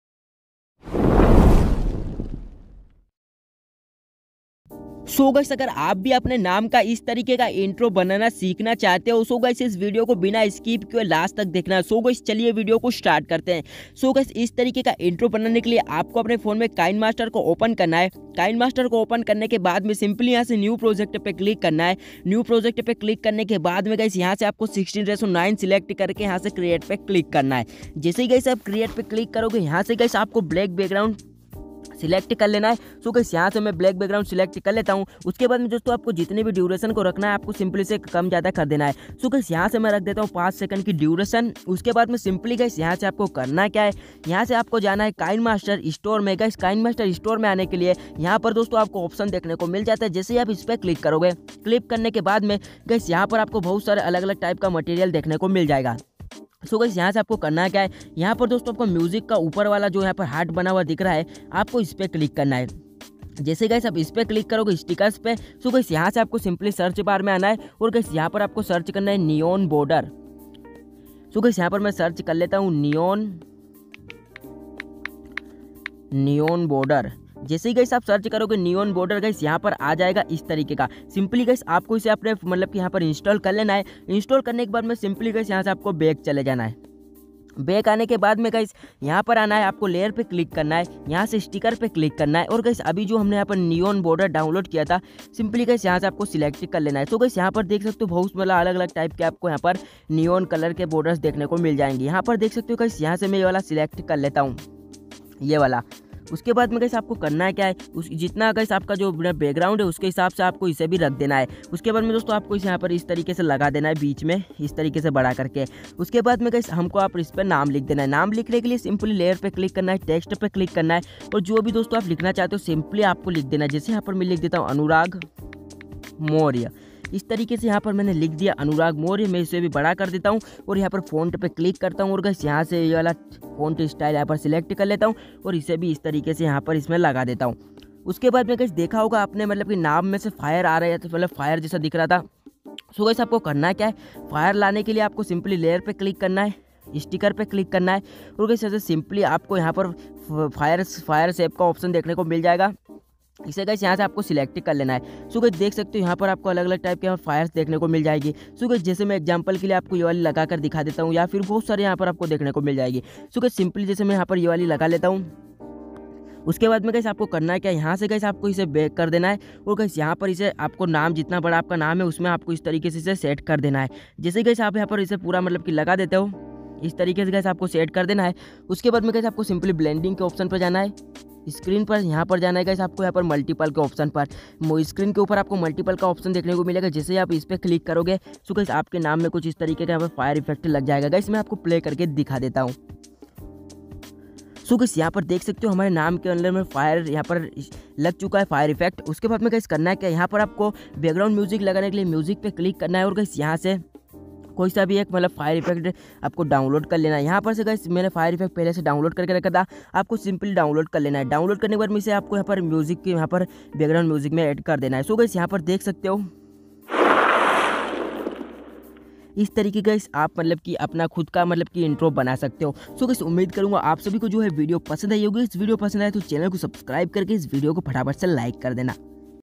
मोबाइल से Intro कैसे बनाएं | How to make Intro for YouTube | Mobile se Intro Kaise banaye Free me
Kinemaster link

Kinmaster se Intro Kaise banaye
Free me Intro Kaise banaye
Mobile Se Intro Kaise banaye
Professional Intro tutorial